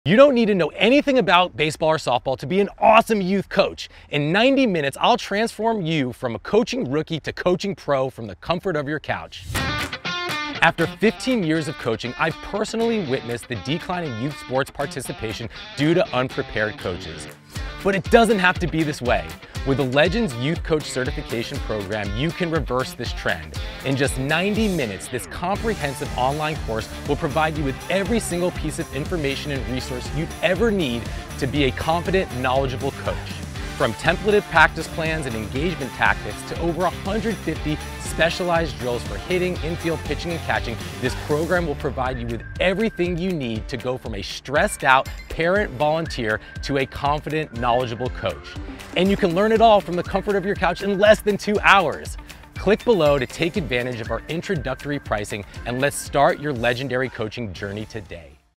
The NEW Youth Coach Training and Certification for Baseball and Softball Coaches!
The NEW Youth Coach Training and Certification program will include training on:
🥎 Arm Care
⚾ Mental game
🥎 Practice planning
⚾ Field Maintenance
🥎 Team Management
⚾Engagement strategies
🥎Recommend equipment
⚾Basic Safety (CPR, AED, etc.)
🥎150+ drills and practice games

You'll never need to search the internet for ideas as I have curated THE BEST drills for youth.

You'll be able to make detailed practice plans in minutes and coach with confidence this season!

It literally covers everything a youth coach could ever need and you can learn it all in less than 2 hours while sitting on your couch!

Im launching it this Thursday night at 6pm PT with discounted pricing.

RSVP to the Launch Party!

And please tag a friend or a league who would benefit from this training!